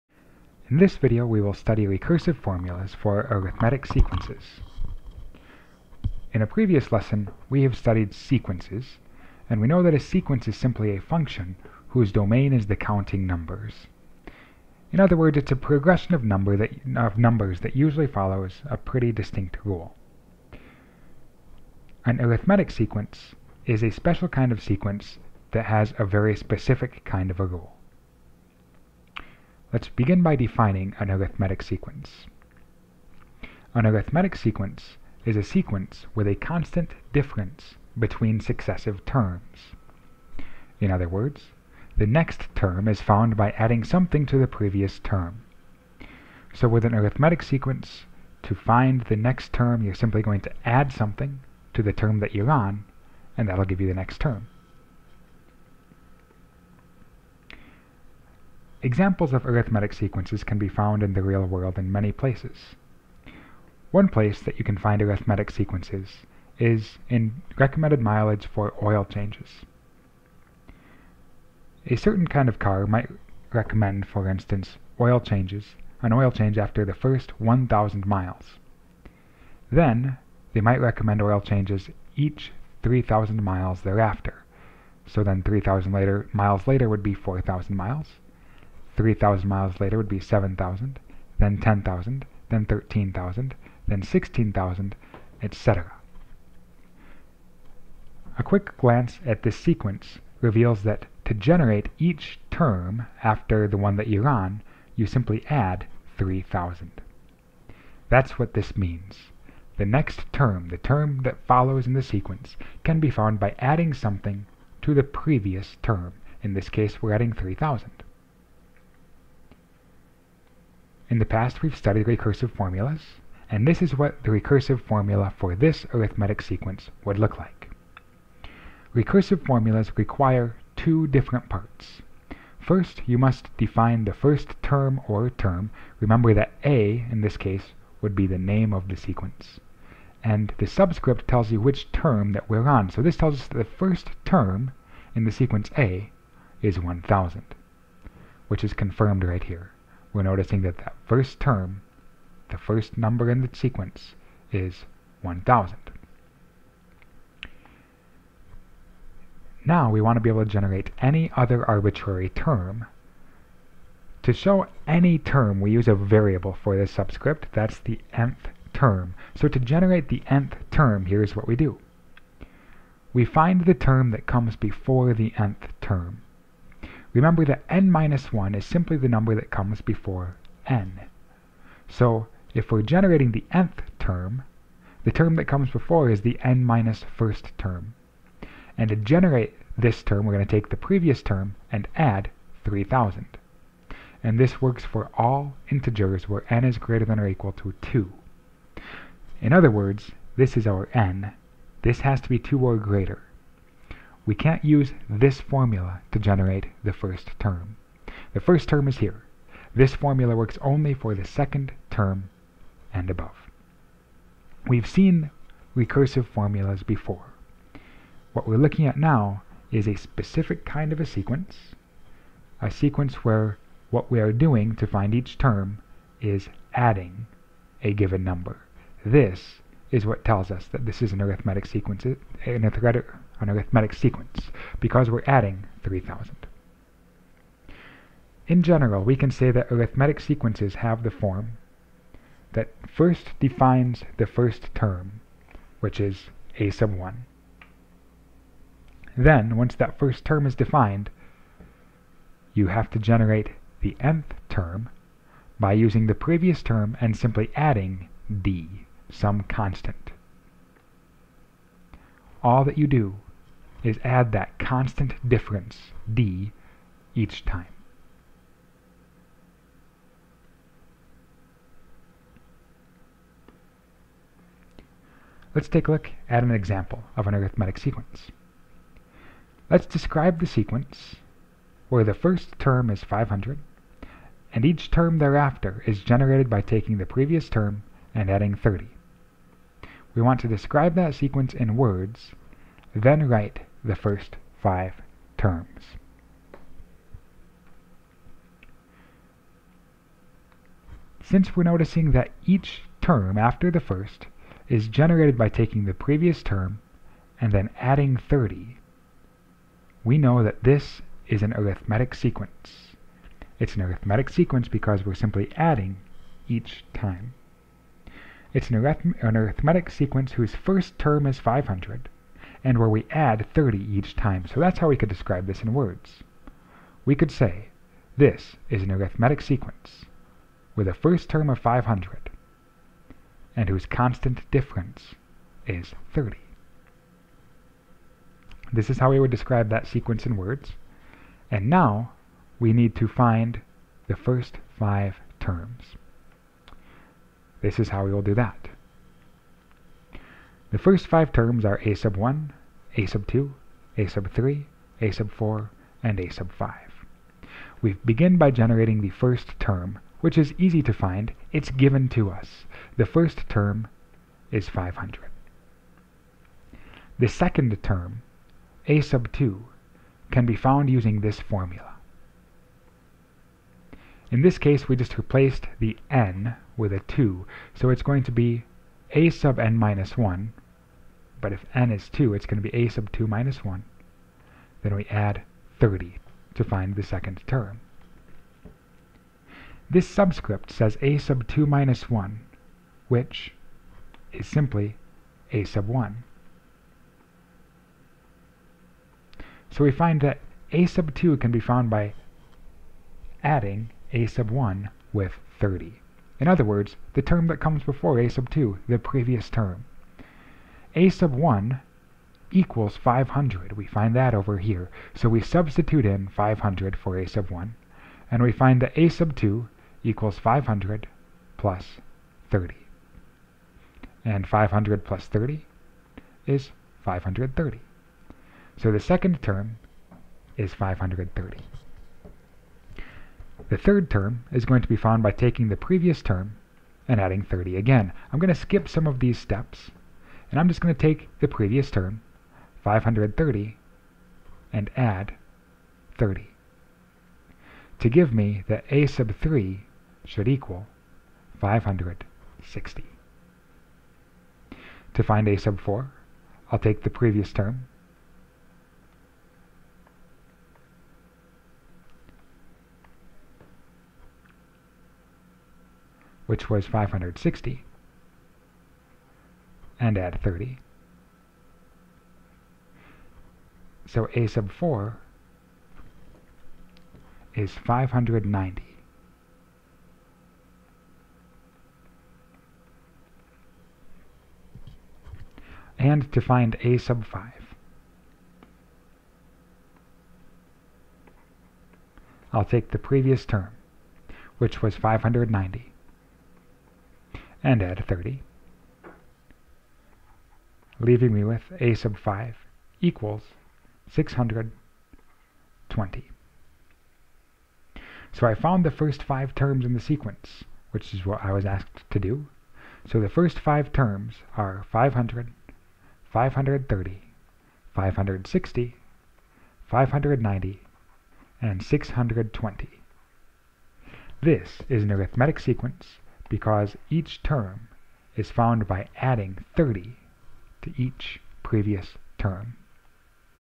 Arithmetic Sequences - Recursive Formula Notes
This video shows how to use recursive formulas to describe arithmetic sequences.